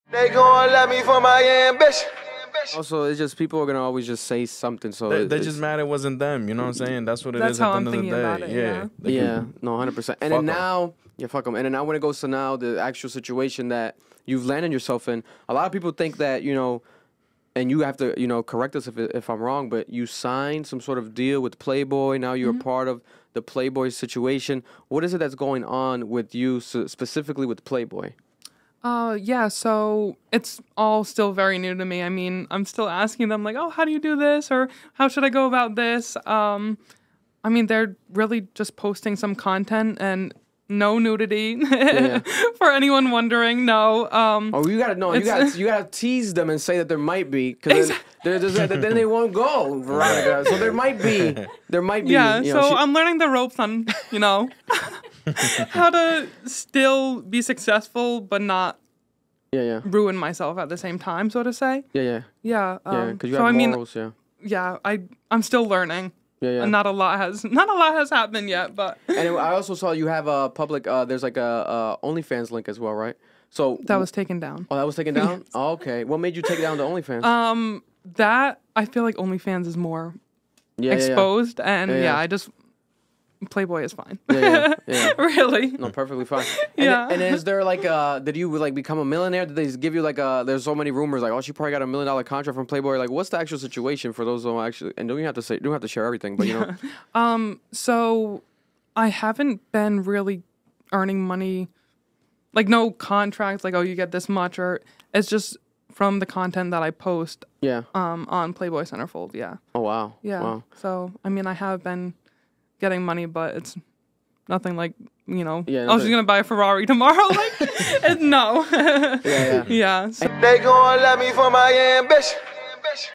36G Drake Bra Girl Explains Her Deal With Playboy | CAP Clips
Want a promo snippet of your song at the beginning of the video?

RIP: Nipsey, Mac, XXXtentacion, Juice, Pop, Von, DMX, Virgil, Dolph, Takeoff